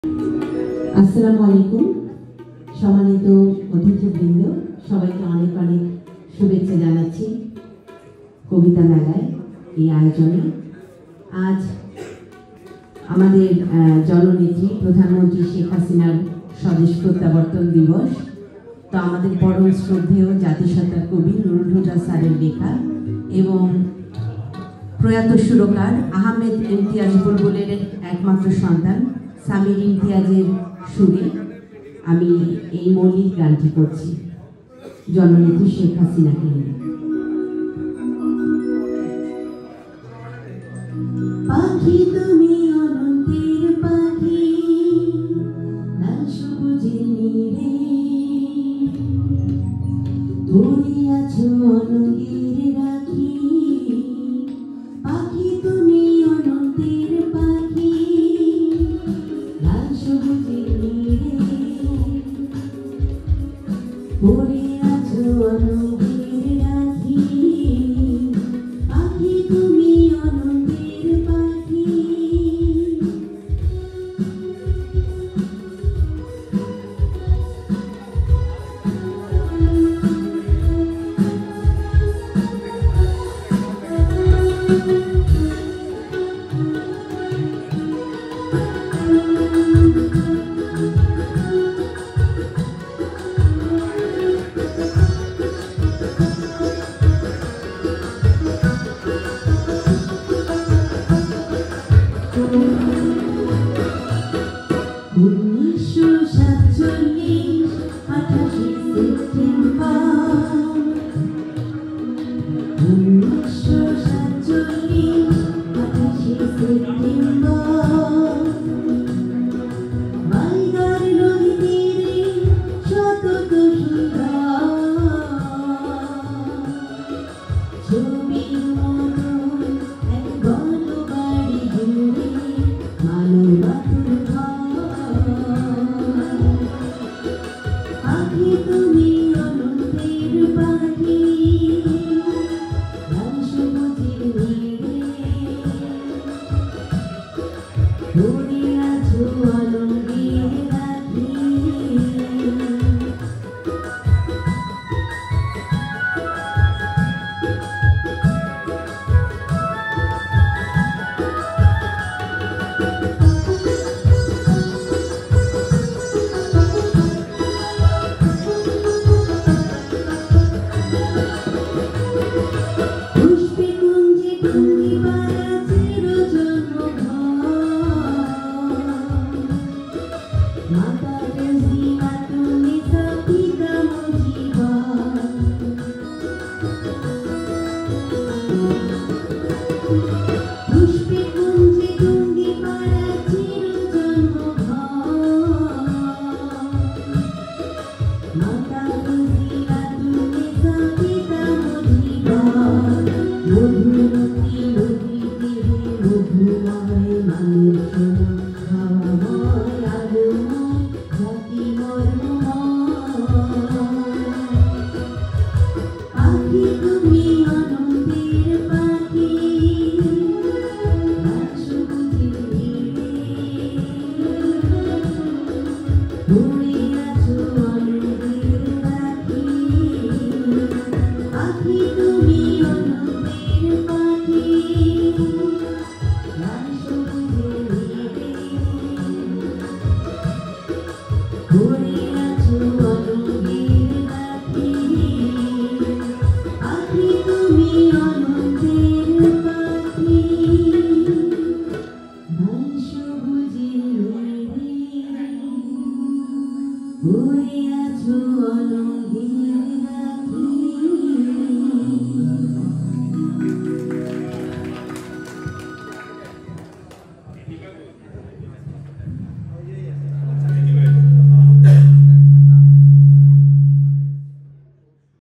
শেখ হাসিনাকে নিবেদন করে রাজিয়া মুন্নীর মৌলিক গান….!
আজ (১৭ ই মে)জননেত্রী শেখ হাসিনার স্বদেশ প্রত্যাবর্তন দিবস ছিল, তাই জাতি সত্তার কবি মুহম্মদ নূরুল হুদার লেখা, সুরকার আহমেদ ইমতিয়াজ বুলবুল স্যারের একমাত্র সন্তান সামির ইমতিয়াজের সুরে জননেত্রী শেখ হাসিনাকে নিবেদন করে এই গানটি গাইলাম। অনুষ্ঠানটি  চট্রগ্রামে অনুষ্ঠিত হয়,  রাইটার্স ক্লাবের আয়োজনে “কবিতা ভ্রমণ”